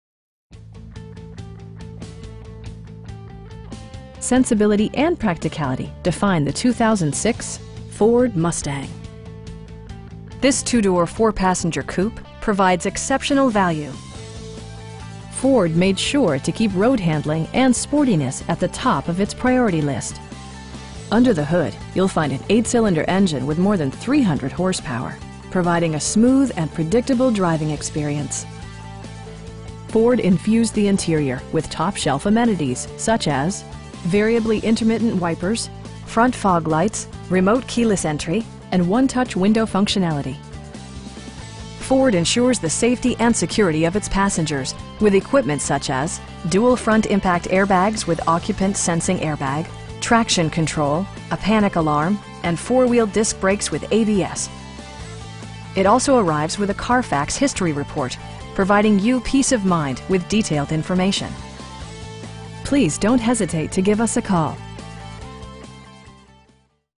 2006 Ford Mustang in Cincinnati, OH 45249
Kings Toyota Scion
4700 Fields Ertel Rd.

Sensibility and practicality define the 2006 Ford Mustang. This 2 door, 4 passenger coupe provides exceptional value! Ford made sure to keep road-handling and sportiness at the top of it's priority list. Under the hood you'll find an 8 cylinder engine with more than 300 horsepower, providing a smooth and predictable driving experience. Ford infused the interior with top shelf amenities, such as: variably intermittent wipers, air conditioning, front fog lights, tilt steering wheel, remote keyless entry, cruise control, and 1-touch window functionality. Ford ensures the safety and security of its passengers with equipment such as: dual front impact airbags with occupant sensing airbag, traction control, a panic alarm, and 4 wheel disc brakes with ABS. It also arrives with a Carfax history report, providing you peace of mind with detailed information. We pride ourselves in the quality that we offer on all of our vehicles. Please don't hesitate to give us a call.